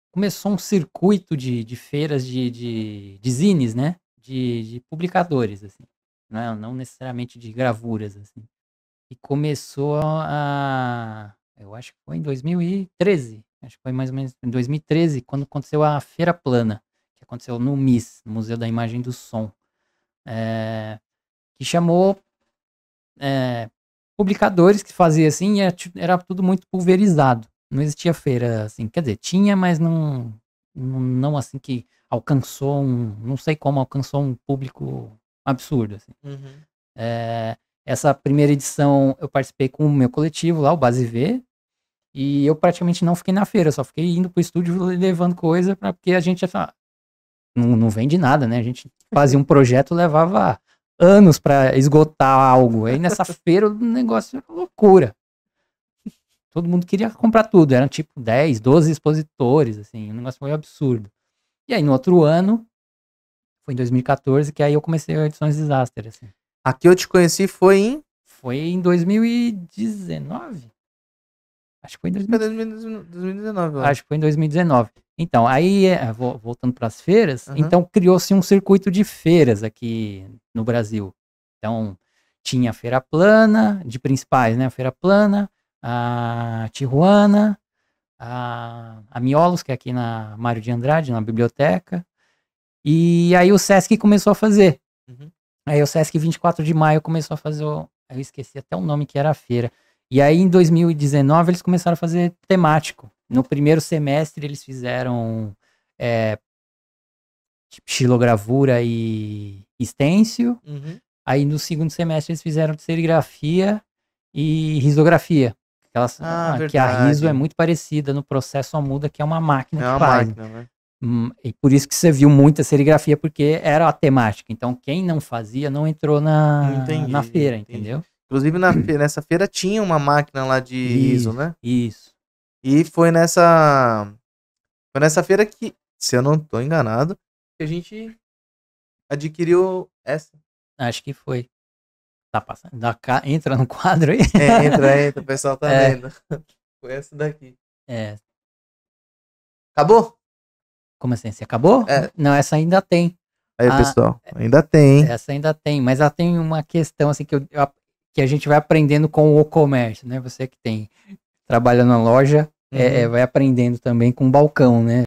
Feiras de Serigrafia.
Cortes do bate papo com o ilustrador Zansky.

👇( ͡▀̿ ̿ ͜ʖ ͡▀̿ ̿ )👇 Só para serígrafos!